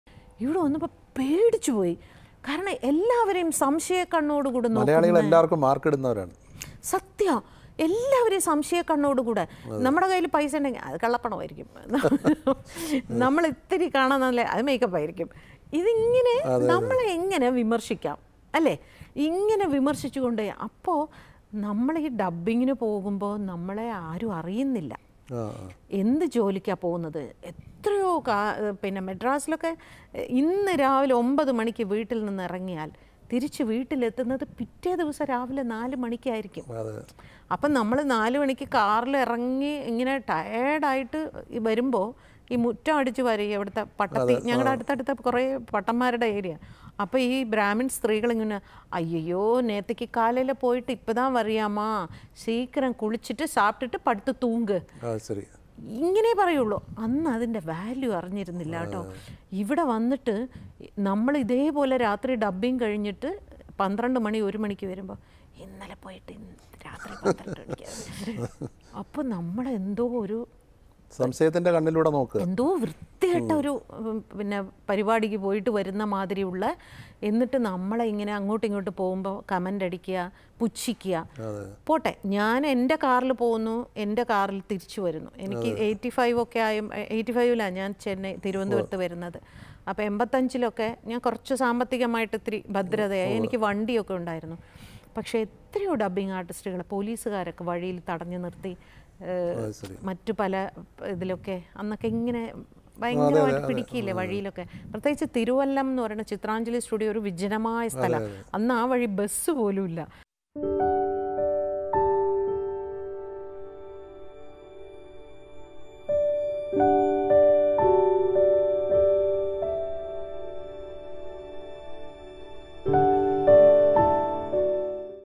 Ugly prying eyes and gossips while returning home after night dubbing | Bhagyalakshmi
Bhagyalakshmi is a leading dubbing artist, actress and an activist. She works primarily in Malayalam Film Industry. She has a dubbed for more than 2000 films in Malayalam cinema. She mostly dubbed for Actress Shobana.